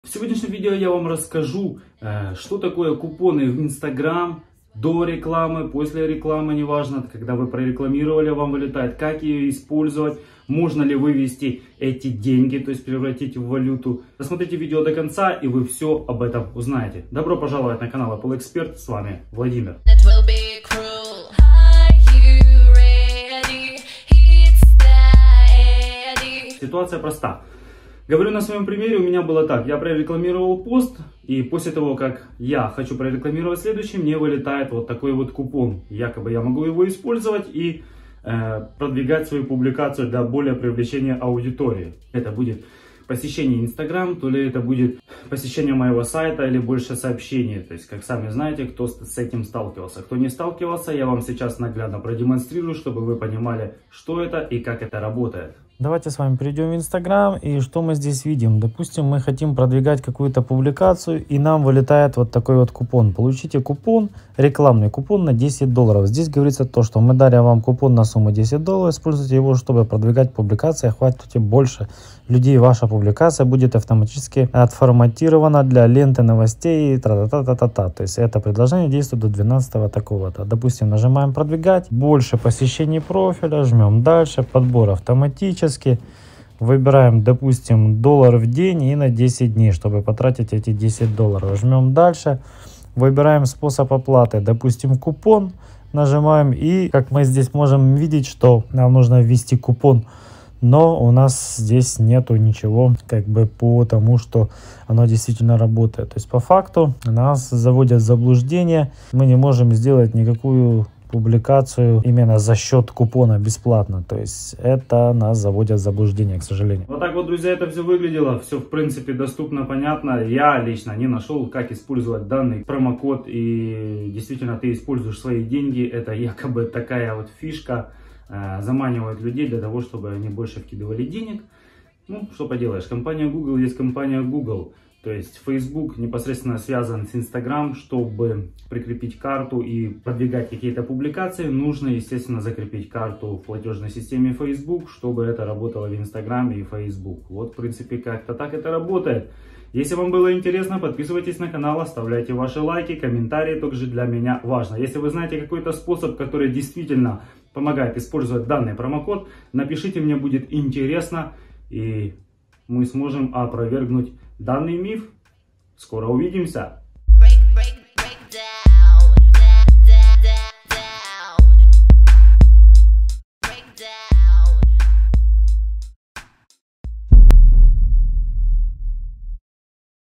Всё о КУПОНАХ INSTAGRAM | КАК ИСПОЛЬЗОВАТЬ рекламный КУПОН | МОЖНО ЛИ ВЫВЕСТИ в ДЕНЬГИ? ​⁠
В данном видео расскажу и покажу на своём примере по купонам instagram.
Можно ли вывести деньги которые дают на рекламу instagram через купон?

Что это за купон instagram?
Что это за рекламный купон instagram?
Как использовать купон instagram?
Как использовать рекламный купон instagram?